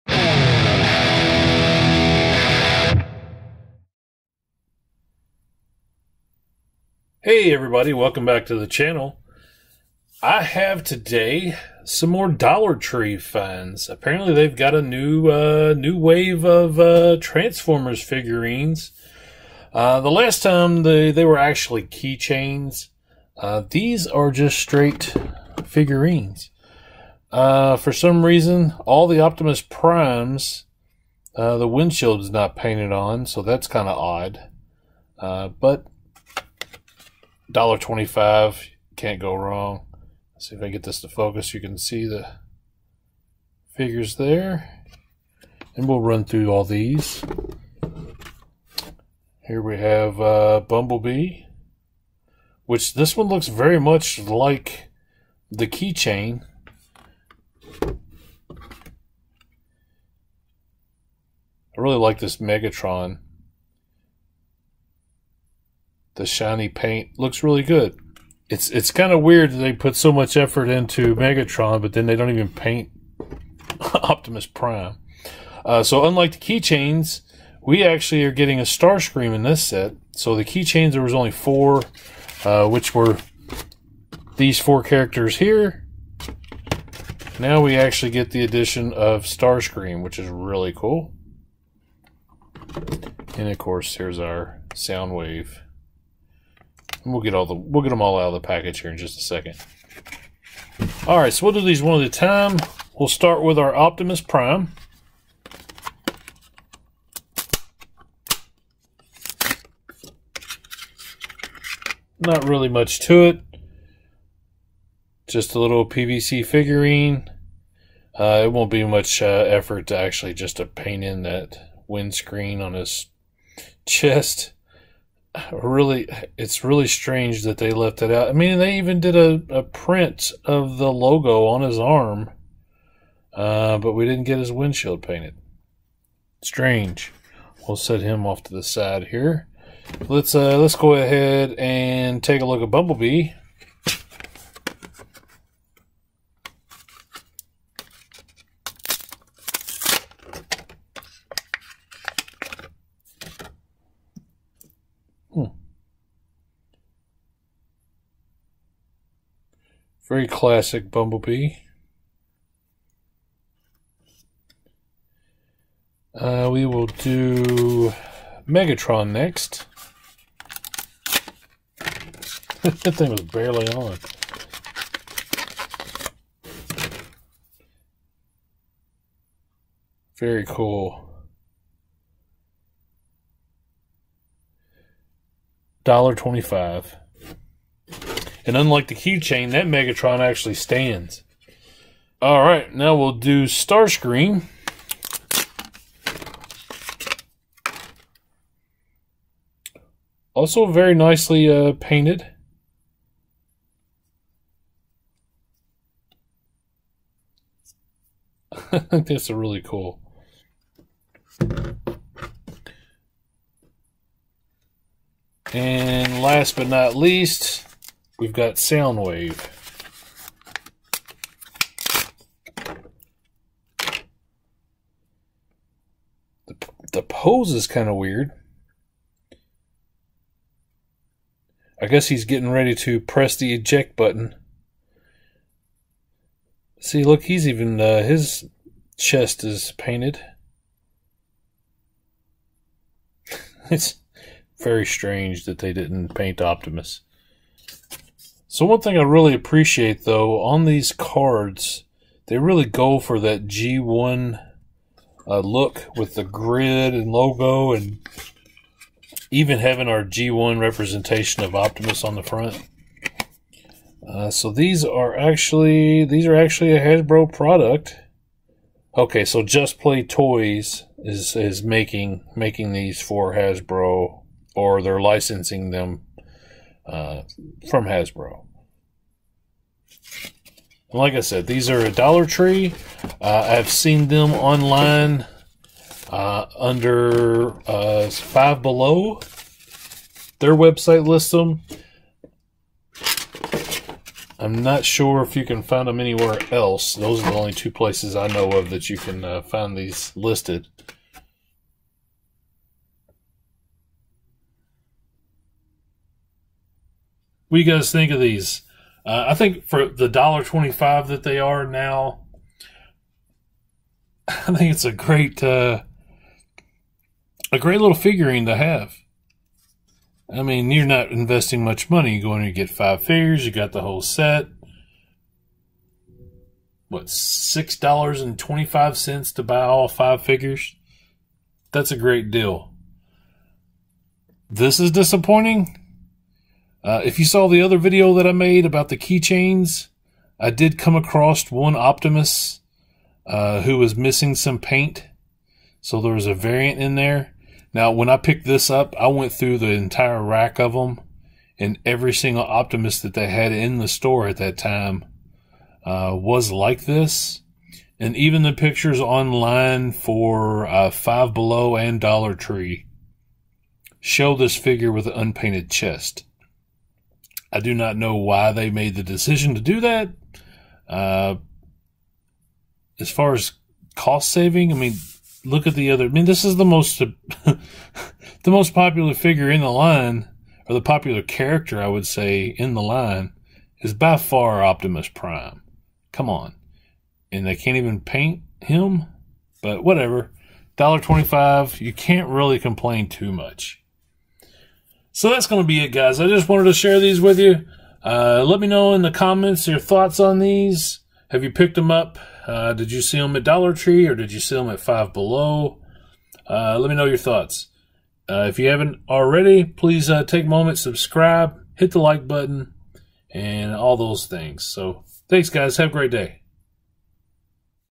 Dollar Tree Transformers Figurines for $1.25
Optimus Prime, Bumblebee, Megatron, Soundwave & Starscream